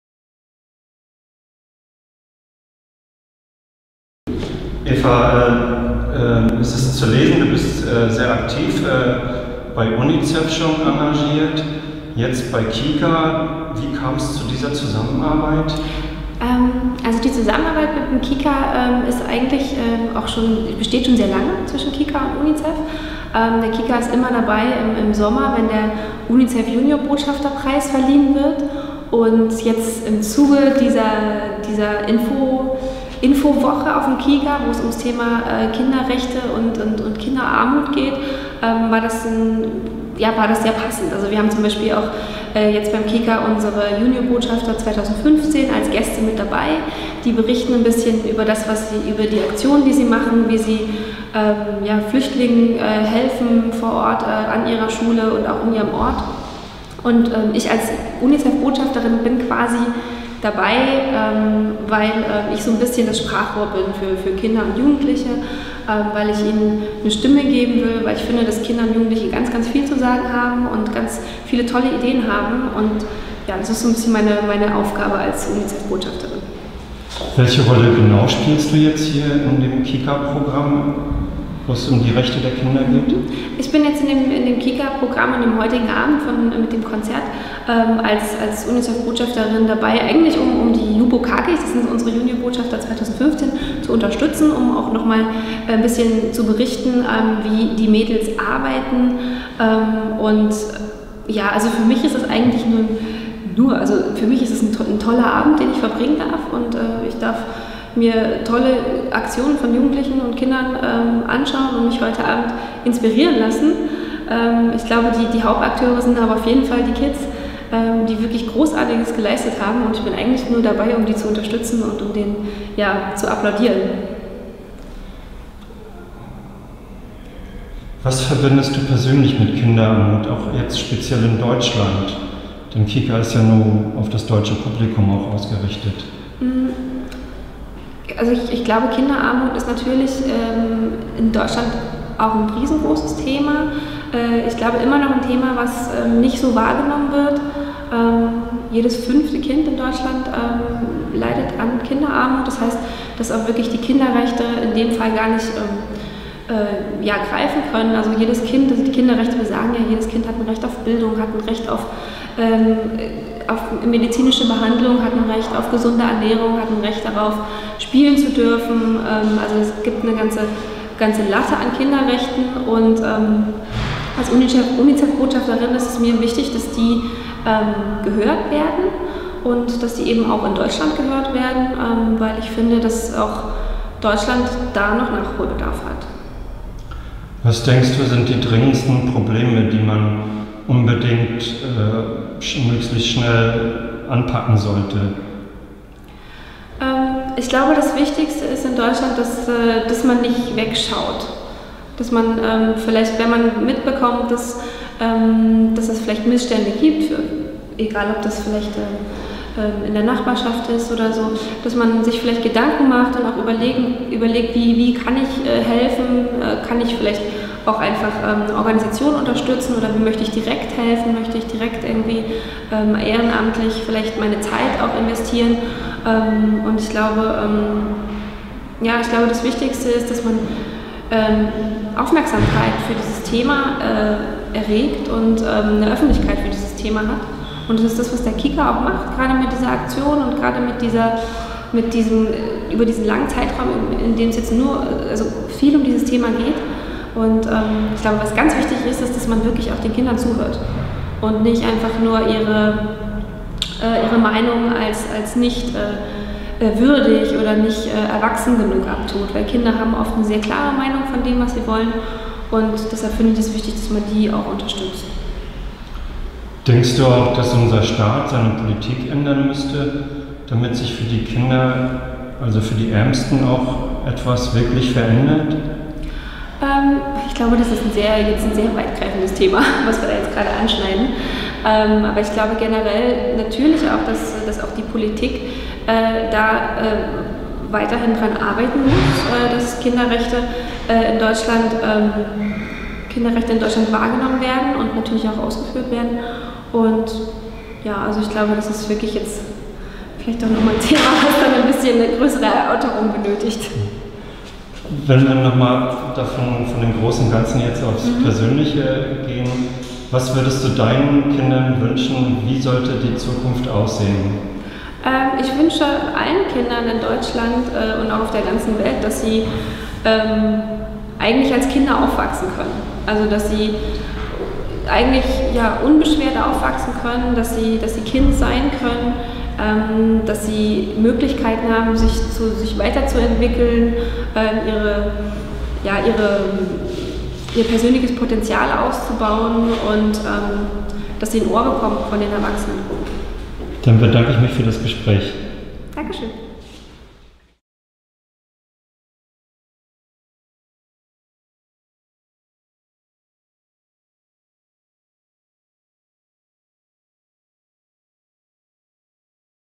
Eva Padberg Im Gespräch
Anläßlich der Aufzeichnung der KIKA-Sendung "Respekt für meine Rechte - Das Konzert" am 5. Oktober 2015 im Berliner E-Werk stellte sich Eva Padberg zum Gespräch über ihr persönliches Engagement gegen Kinderarmut und für Kinderrechte.